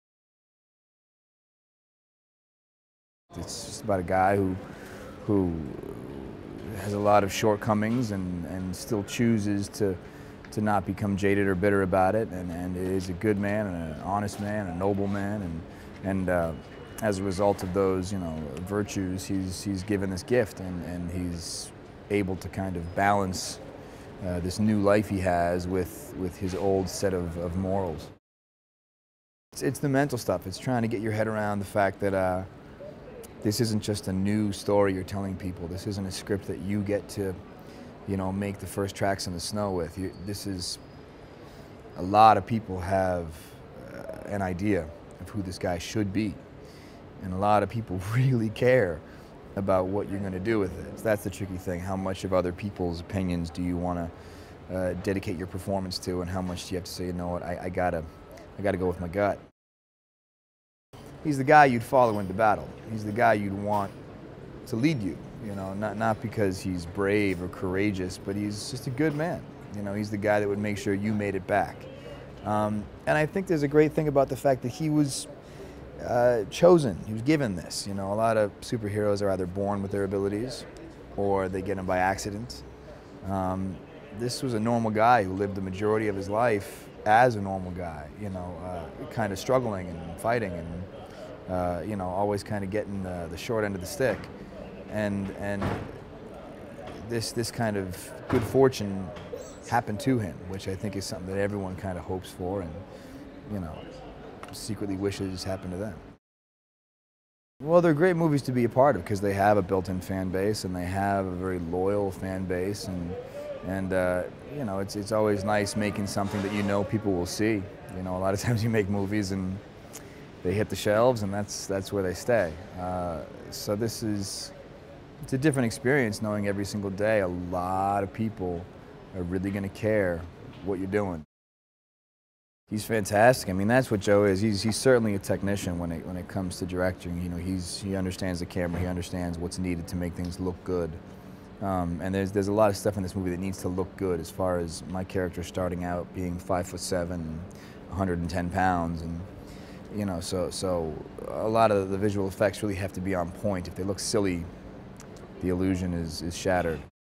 Entrevista a Chris Evans. Protagonista de "Capitán América: El primer Vengador"
Con el fichaje clave del protagonista principal los productores de "Capitán América" comenzaron el casting con buen pie. CHRIS EVANS (Steve Rogers/Capitán América) ha surgido recientemente como uno de los actores más demandados de Hollywood, tanto en proyectos de gran presupuesto como en filmes independientes. Evans, al principio, experimentó cierta reticencia cuando le hablaron del proyecto. El alcance y la magnitud del compromiso le dio un poco de vértigo.  Para ayudar a aliviar sus preocupaciones, los cineastas le hablaron a Evans del personaje, no de la película de acción, centrándose en quién es Steve Rogers.